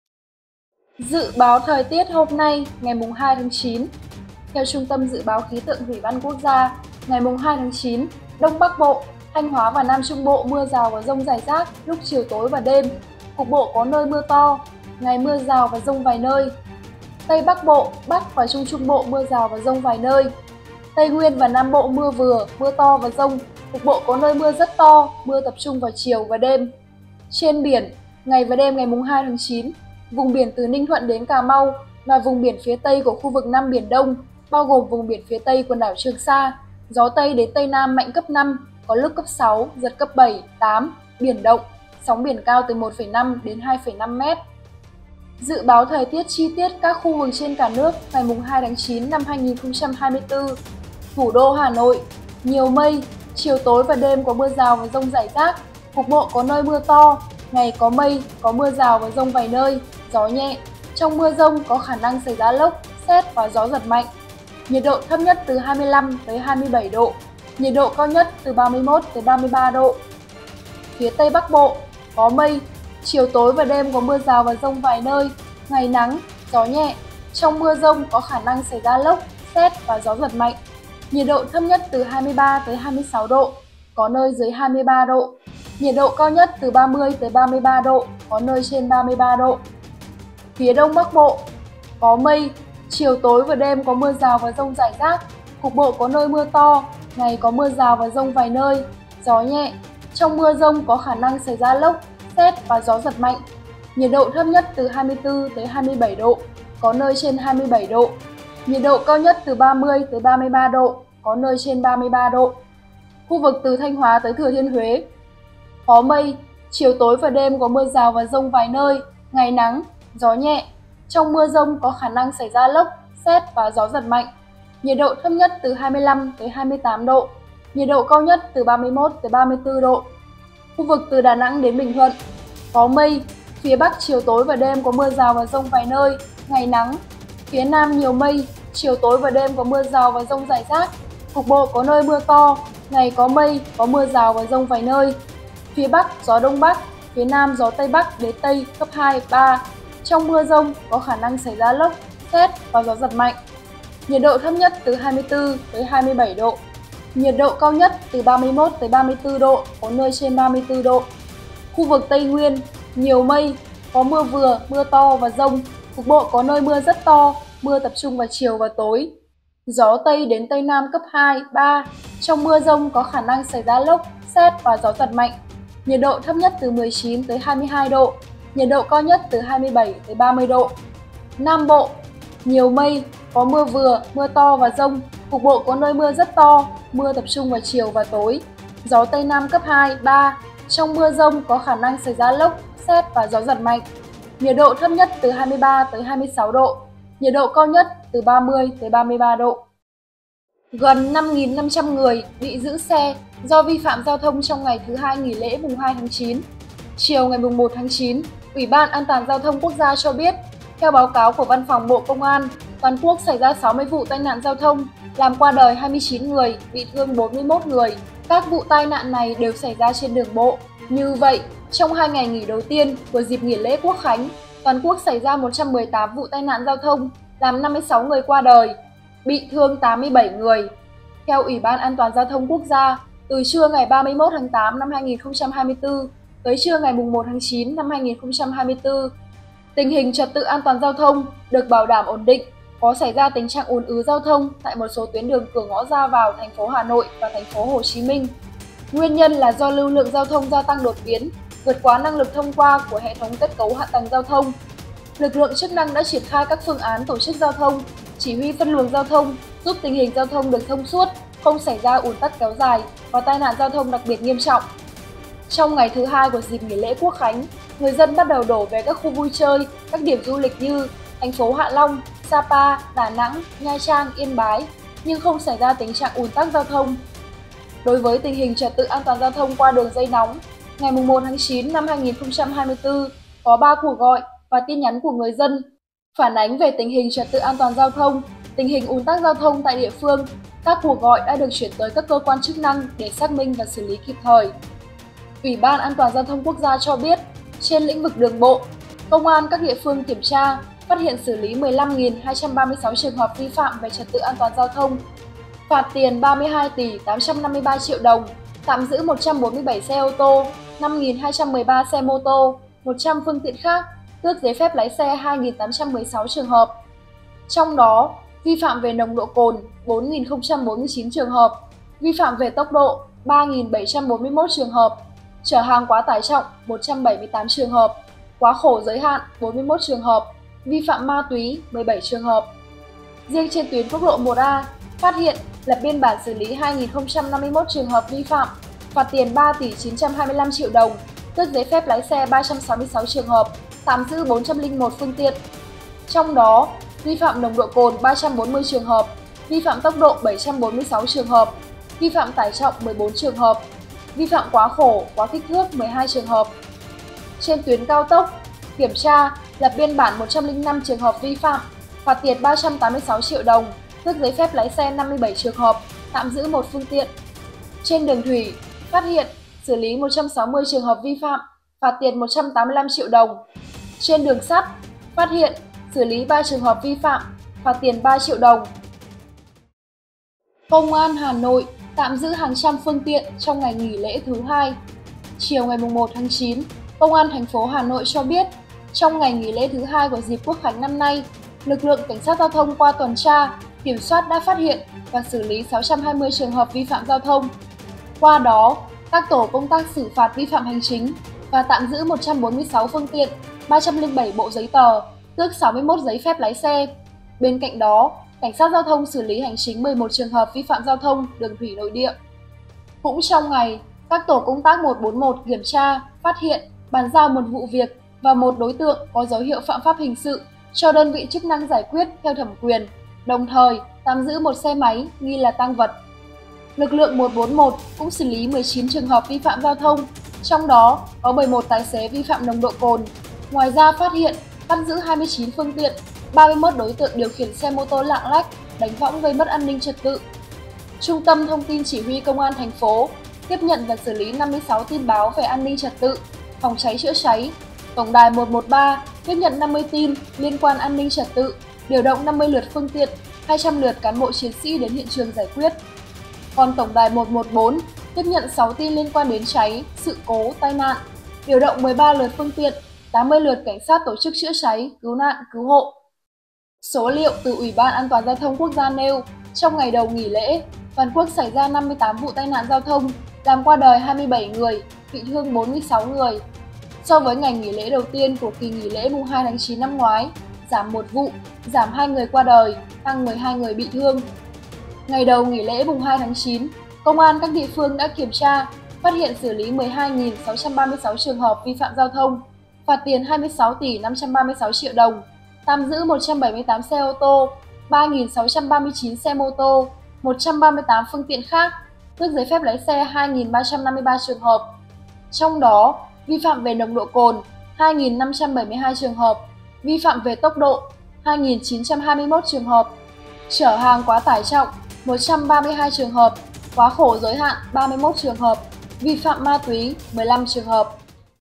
Dự báo thời tiết hôm nay ngày 2/9 | BGT
BGT | Bản tin dự báo thời tiết hôm nay cập nhật tình hình thời tiết, khí hậu mới nhất hôm nay 02/09/2024

- Địa chỉ tòa soạn: Số 2, Nguyễn Công Hoan, Ba Đình, Hà Nội